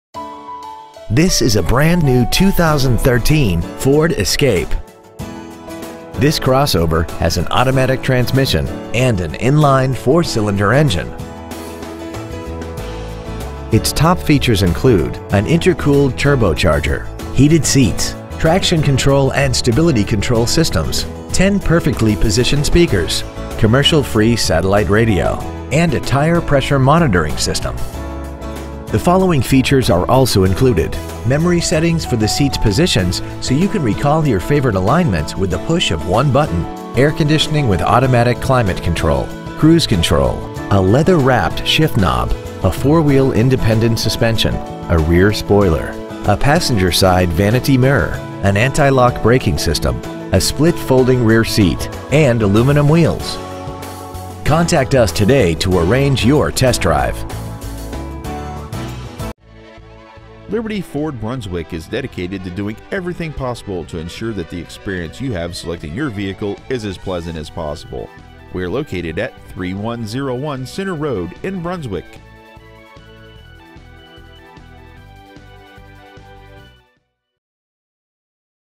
2013 Ford Escape Brunswick OH
Year : 2013
 Make : Ford
 Model : Escape
 Engine : 2.0L I4 16V GDI DOHC TURBO
 Trans . : 6-SPEED AUTOMATIC
 Exterior : TUXEDO BLACK
 Miles : 4
 Interior : CHARCOAL BLACK
 Stock : TM60725